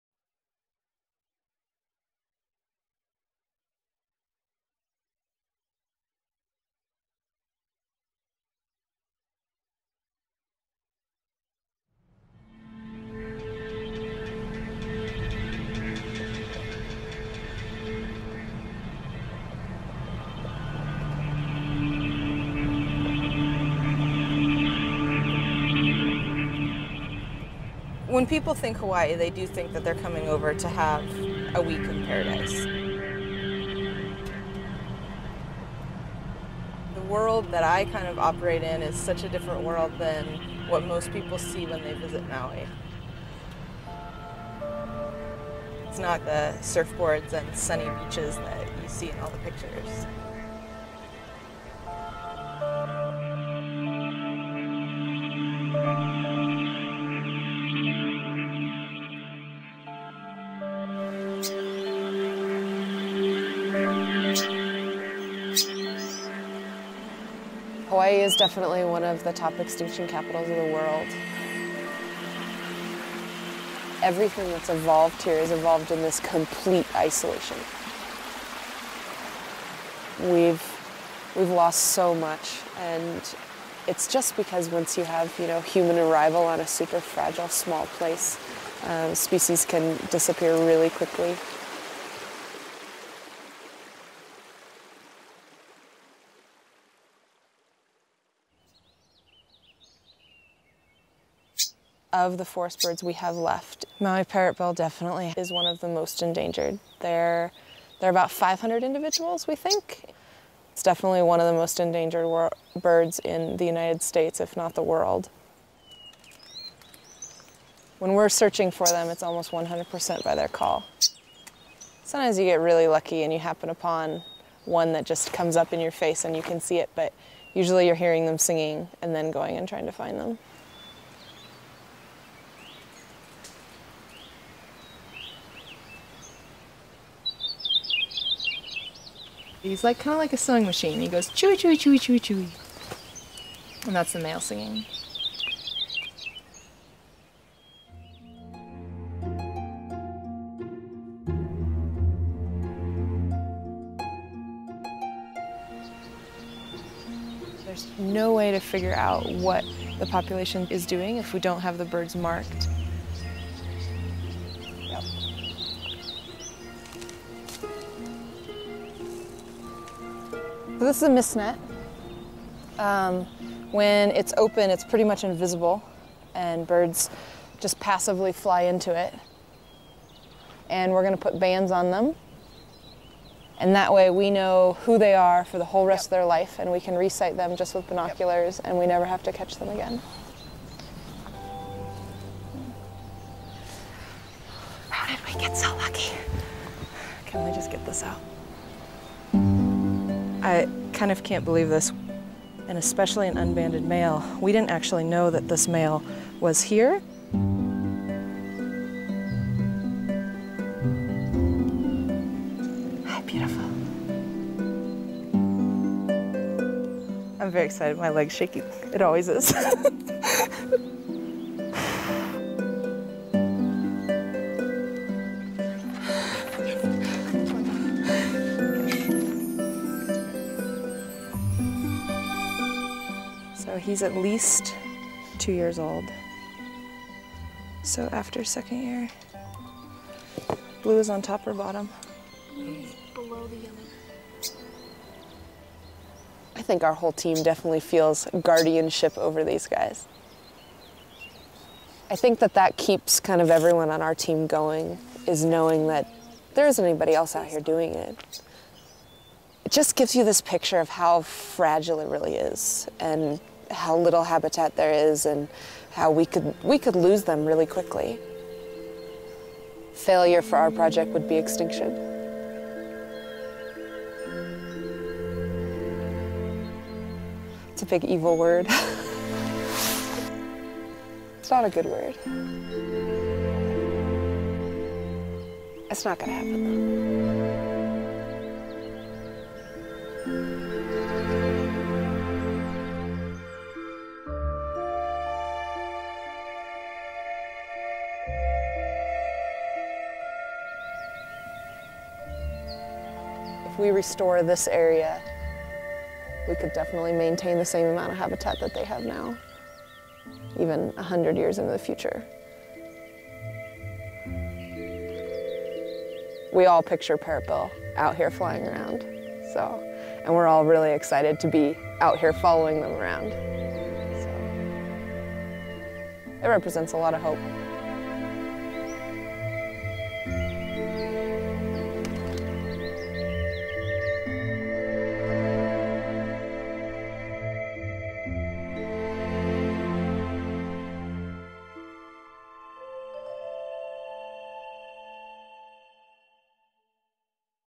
Maui Parrotbill Conservation
A short documentary about preserving the Maui Parrotbill in Hawaii featuring the conservation work of Hanna Mounce